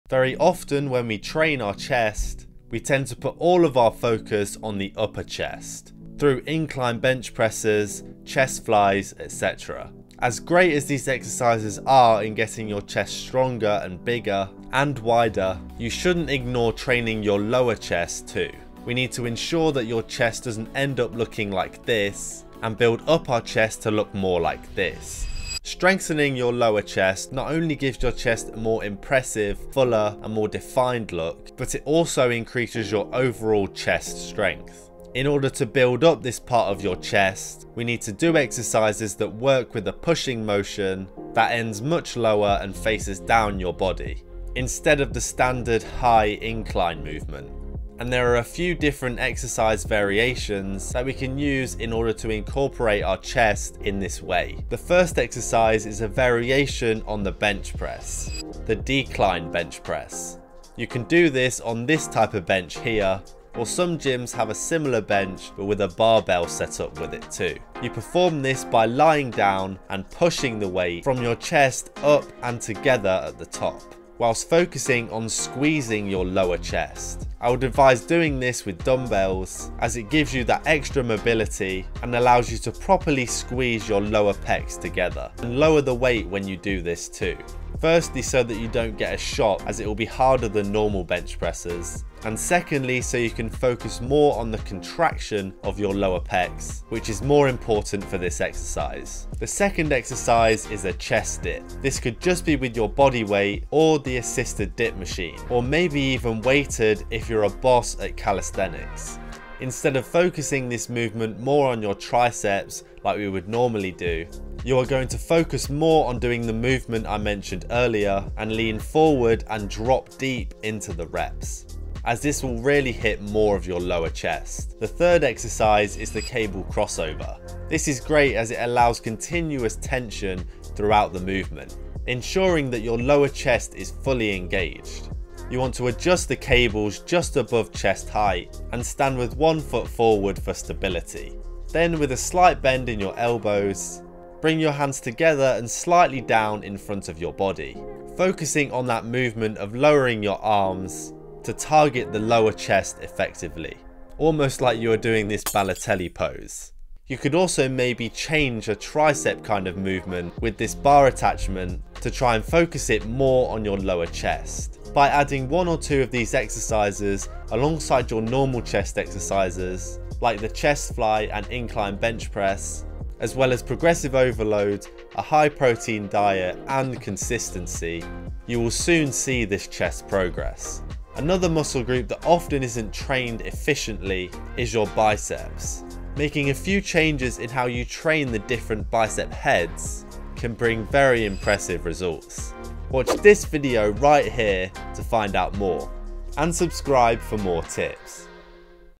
How to Build Muscle On Your LOWER CHEST - 3 EASY Exercises!
In this video I go over how you can transform your lower chest with a few easy exercises. Building muscle on your lower chest is very important in getting a bigger chest and a more defined looking chest. But many of you struggle with this due to only focusing on upper chest exercises. These lower chest exercises will help you build a more defined lower chest, give you more of a chest line and grow you more mass. Doing these easy exercises alongside your other chest exercises like bench press, chest fly etc. will give you a chest transformation!

💪 Some useful HOME WORKOUT equipment I recommend buying:

Push up board
Resistance bands
Pull up bar
Grip Strengthener
Punch Bag
Skipping rope

Chapters:

0:00 - Why Is It Important To Train Our Lower Chest And How?
0:55 - Exercise 1
1:40 - Exercise 2
2:05 - Exercise 3 & 4

Stock footage: Pixabay, Videezy, coverr, pexels